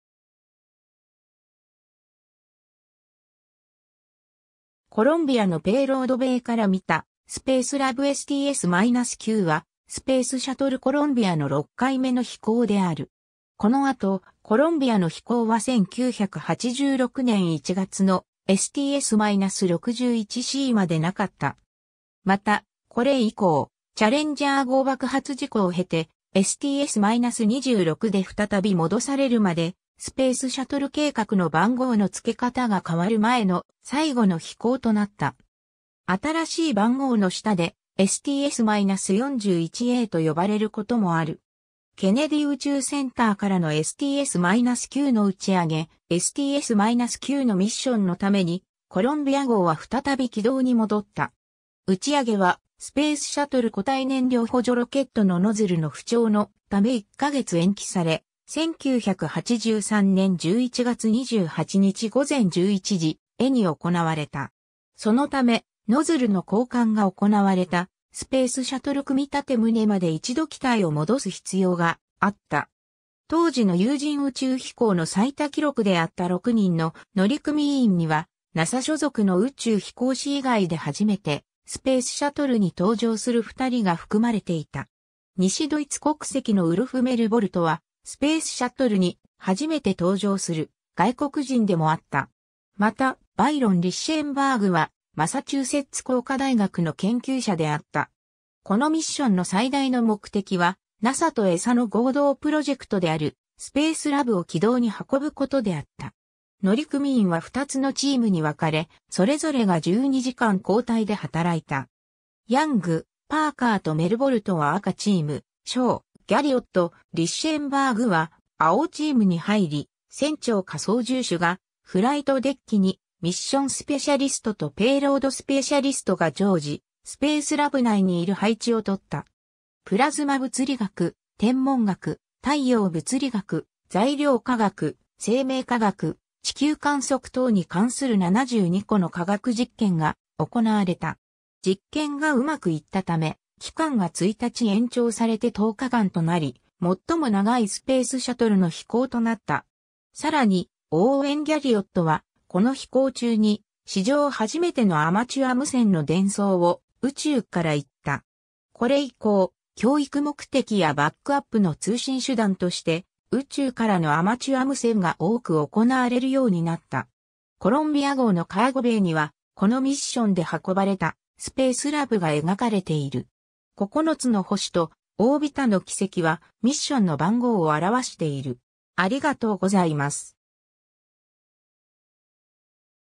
STS-9
コロンビアのペイロードベイから見たスペースラブ STS-9は、スペースシャトルコロンビアの6回目の飛行である。
この後、コロンビアの飛行は1986年1月のSTS-61-Cまで無かった。
また、これ以降、チャレンジャー号爆発事故を経てSTS-26で再び戻されるまで、スペースシャトル計画の番号の付け方が変わる前の最後の飛行となった。
新しい番号の下で、STS-41-Aと呼ばれることもある。
 ケネディ宇宙センターからのSTS-9の打上げ STS-9のミッションのために、コロンビア号は再び軌道に戻った。
打上げは、スペースシャトル固体燃料補助ロケットのノズルの不調のため1ヶ月延期され、1983年11月28日午前11時ESTに行われた。
そのため、ノズルの交換が行われたスペースシャトル組立棟まで一度機体を戻す必要があった。
 当時の有人宇宙飛行の最多記録であった6人の乗組員には、NASA所属の宇宙飛行士以外で初めてスペースシャトルに搭乗する2人が含まれていた。
西ドイツ国籍のウルフ・メルボルトは、スペースシャトルに初めて登場する外国人でもあった。
また、バイロン・リッシェンバーグはマサチューセッツ工科大学の研究者であった。
 このミッションの最大の目的は、NASAとESAの合同プロジェクトであるスペースラブを軌道に運ぶことであった。
 乗組員は2つのチームに分かれ、それぞれが12時間交代で働いた。
ヤング、パーカーとメルボルトは赤チーム、ショウ、ギャリオット、リッシェンバーグは青チームに入り、船長か操縦手がフライトデッキに、ミッションスペシャリストとペイロードスペシャリストが常時スペースラブ内にいる配置を取った。
 プラズマ物理学、天文学、太陽物理学、材料科学、生命科学、地球観測等に関する72個の科学実験が行われた。
実験がうまくいったため、期間が1日延長されて10日間となり、最も長いスペースシャトルの飛行となった。
 さらに、オーウェン・ギャリオットはこの飛行中に、史上初めてのアマチュア無線の伝送を宇宙から行った。
これ以降、教育目的やバックアップの通信手段として宇宙からのアマチュア無線が多く行われるようになった。
 コロンビア号のカーゴベイには、このミッションで運ばれたスペースラブが描かれている。
9つの星とオービタの軌跡は、ミッションの番号を表している。